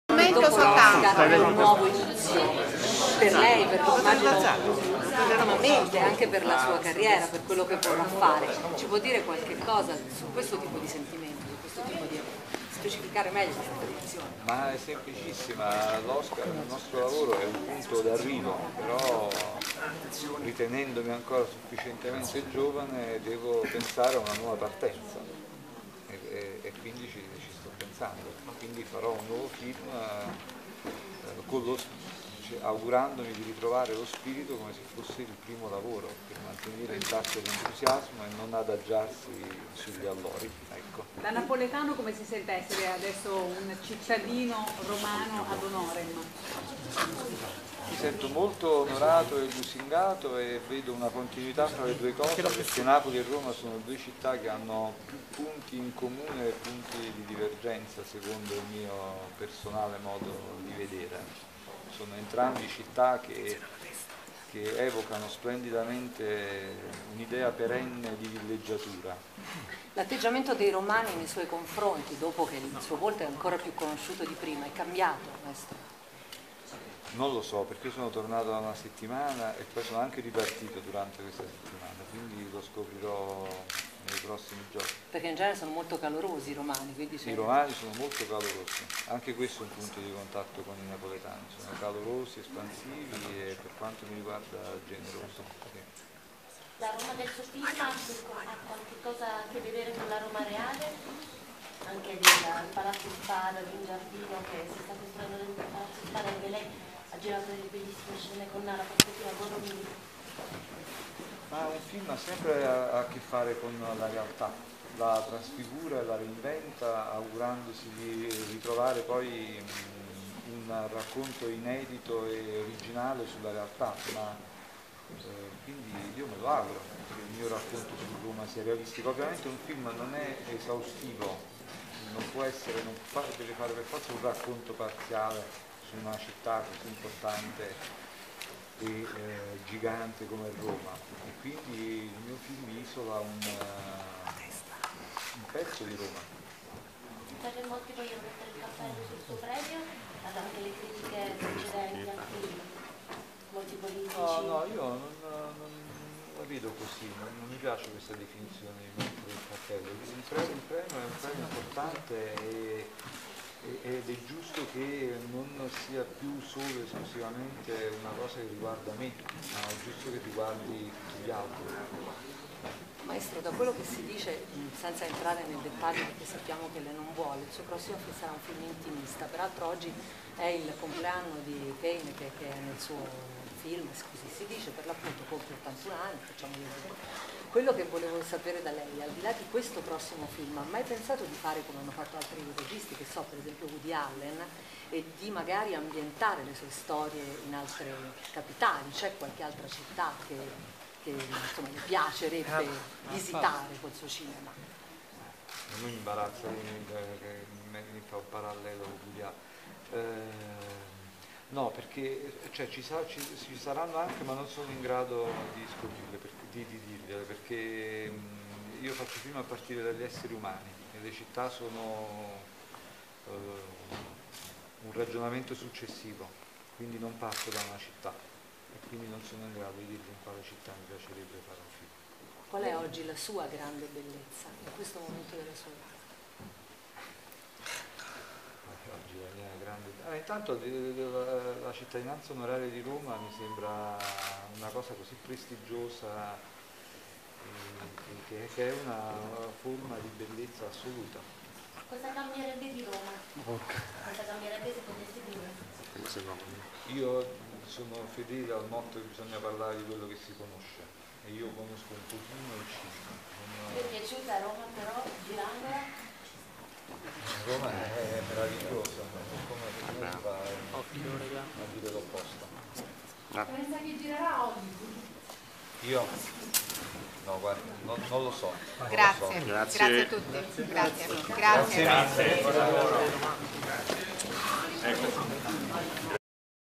Oscar La Grande Bellezza Film di Paolo Sorrentino - Conferenza in Campidoglio
Il regista del film Oscar 2014 La Grande Bellezza, Paolo Sorrentino, in conferenza stampa a Roma nel campidoglio dopo la cerimonia onoraria in suo favore presieduta dal Sindaco di Roma Ignazio Marino.